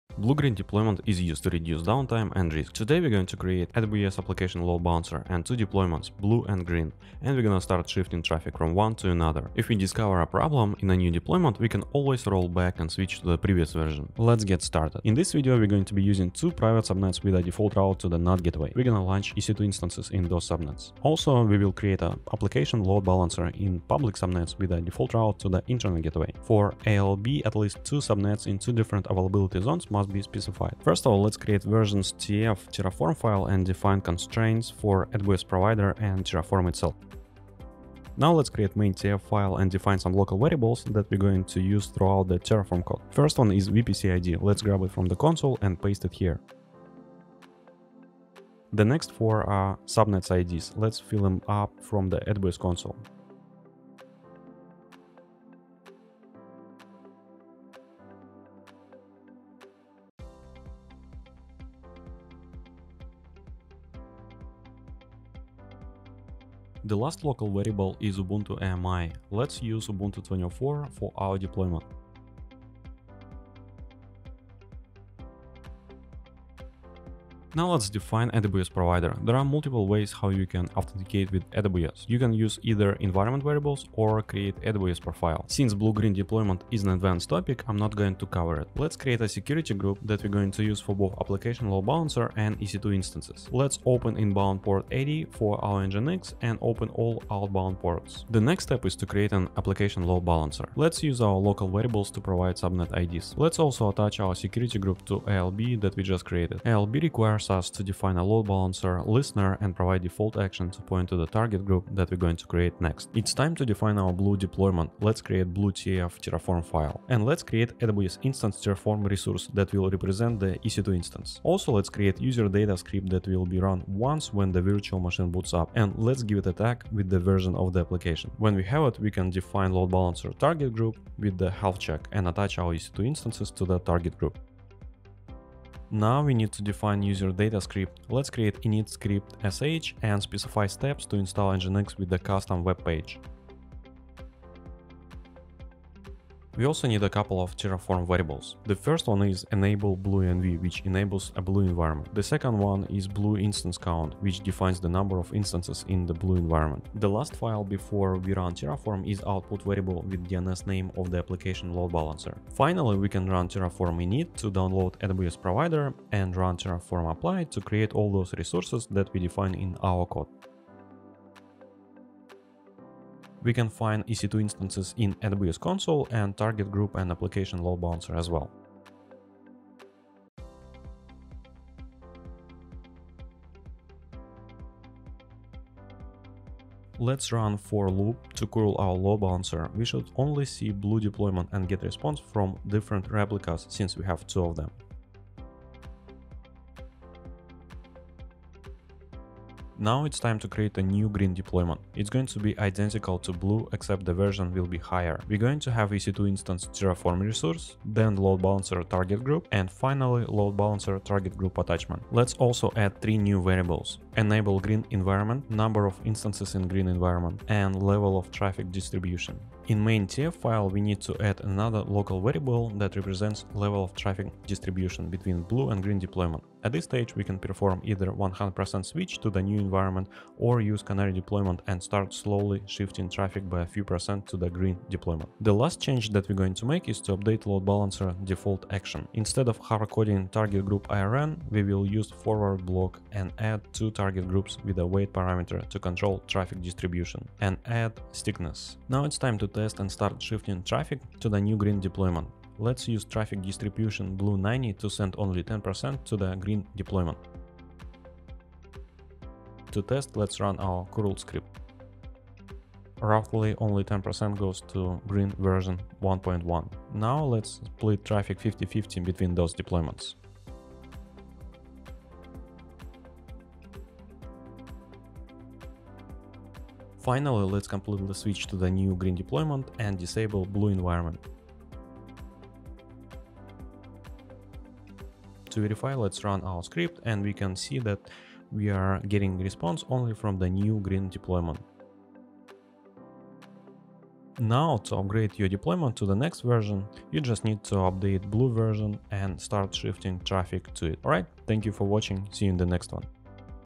Terraform Blue Green Deployment & Terraform Canary Deployment 🔵🐣🟢 AWS | ALB | EC2
⏱️TIMESTAMPS⏱️
0:00 Intro
0:15 AWS Networking Overview
0:30 Create versions.tf
0:38 Create main.tf
1:52 Create blue.tf
2:16 Create init-script.sh
2:26 Create variables.tf
2:36 Create output.tf
2:42 Deploy Blue Environment
3:20 Create green.tf
4:08 Create and Shift 10% of traffic to Green Deployment
4:27 Split traffic 50/50 between Blue and Green Deployment
4:37 Switch 100% of traffic to new Green Deployment and Destroy Blue